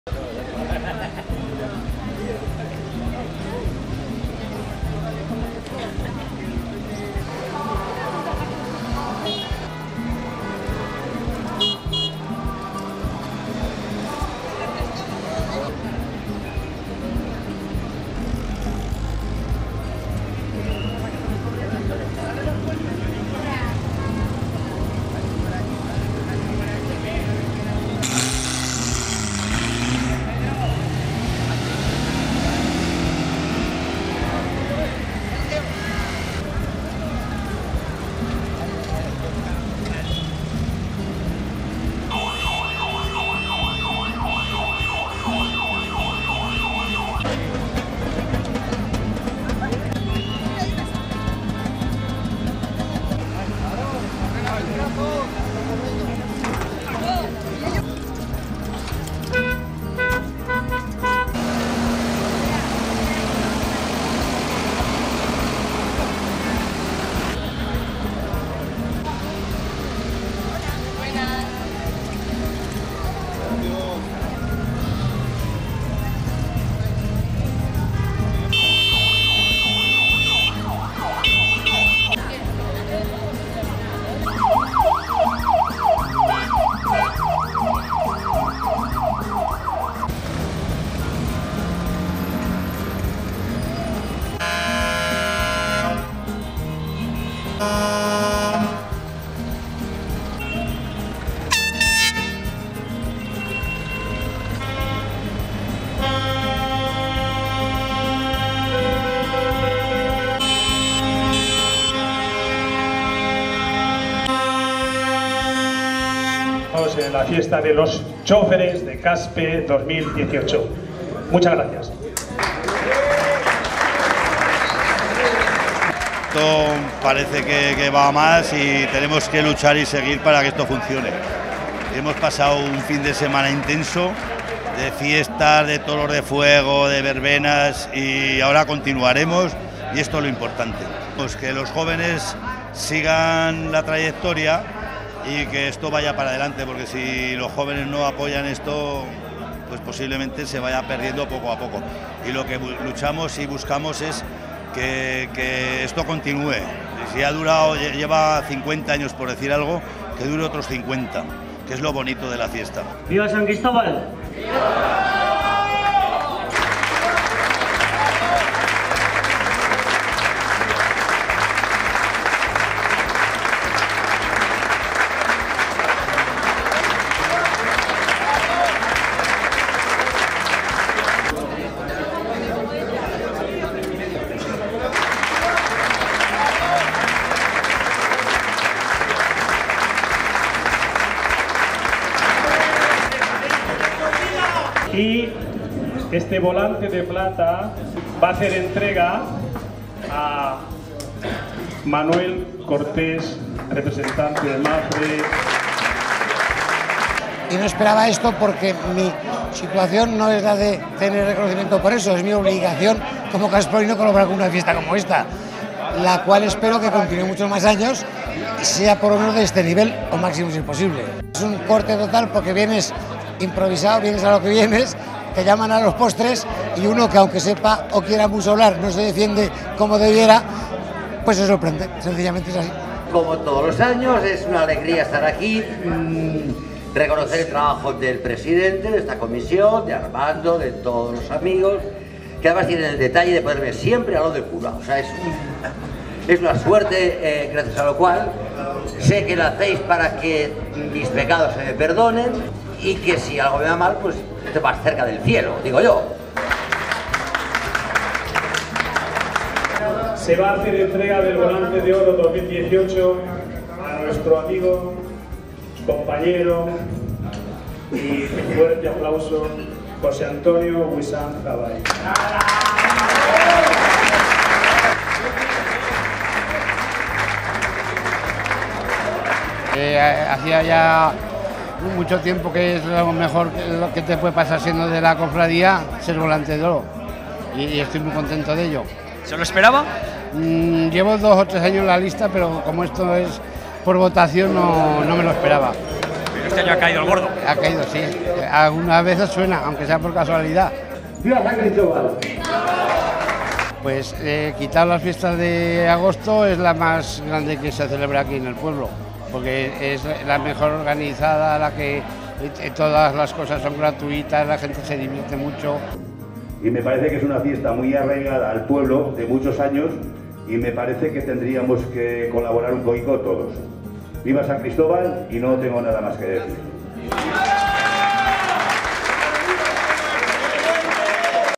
San Cristobal Caspe 2018
También se ha celebrado la festividad de San Cristobal en Caspe. En este reportaje podemos ver como transcurrió la jornada en la ciudad del compromiso, desde la bendición de vehículos hasta la entrega de los volantes de Plata y Oro.